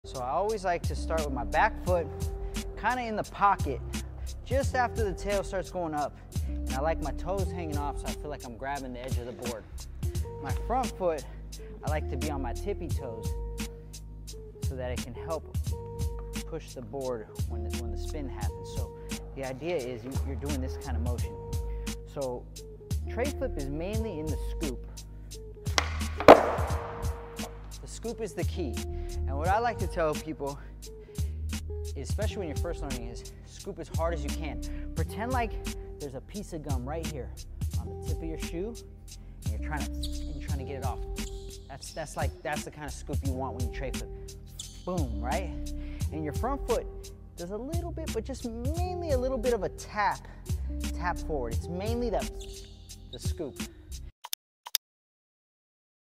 Learn 360 Flip from Paul Rodriguez#short
Paul Rodriguez gives tips and tricks on learning the 360 flip. Watch and enjoy.